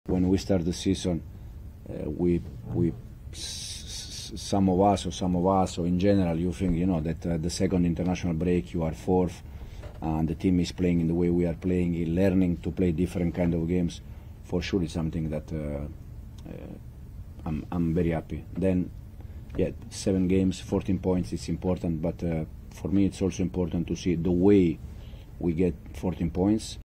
MARESCA CONTENTO con el RENDIMIENTO del CHELSEA en la PREMIER LEAGUE
Enzo Maresca se ha mostrado conforme con el rendimiento mostrado por el Chelsea hasta ahora en la Premier League. Esto ha dicho.